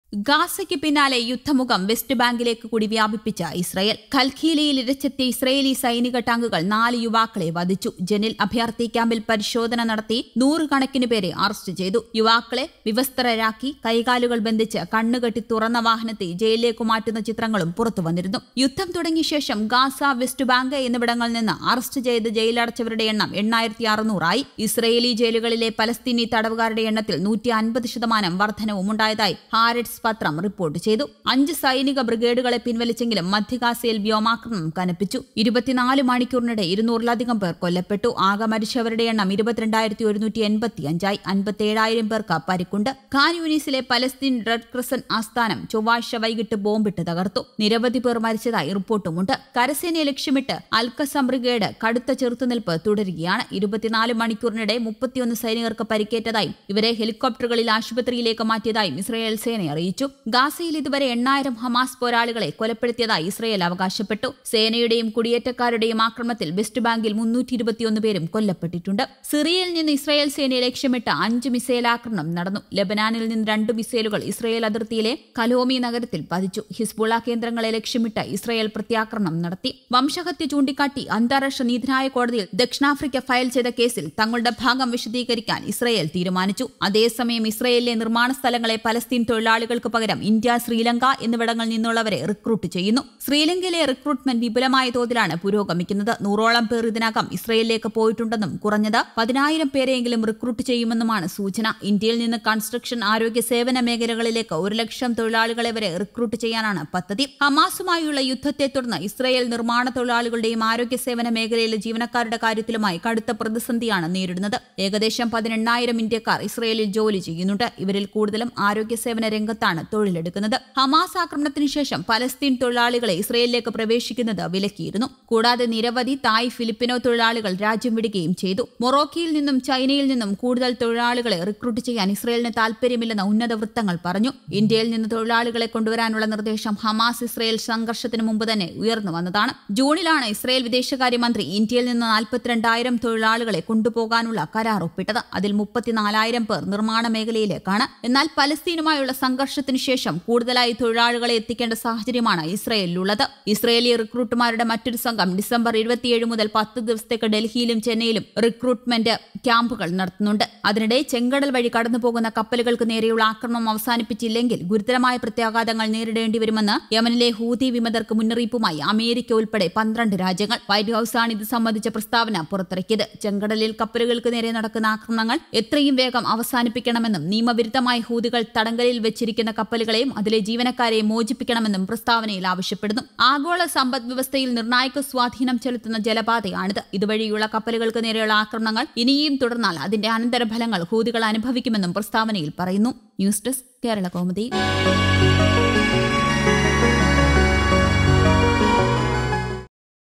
യുദ്ധമുഖം വെസ്റ്റ്ബാങ്കിലേക്കും വ്യാപിപ്പിച്ച് ഇസ്രായേല്‍ | Israel News | Gaza
ഗസ്സക്ക് പിന്നാലെ യുദ്ധമുഖം വെസ്റ്റ്ബാങ്കിലേക്കു കൂടി വ്യാപിപ്പിച്ച് ഇസ്രായേല്‍. ഖല്‍ഖീലിയയില്‍ ഇരച്ചെത്തിയ ഇസ്രായേലി സൈനിക ടാങ്കുകള്‍ നാല് യുവാക്കളെ വധിച്ചു.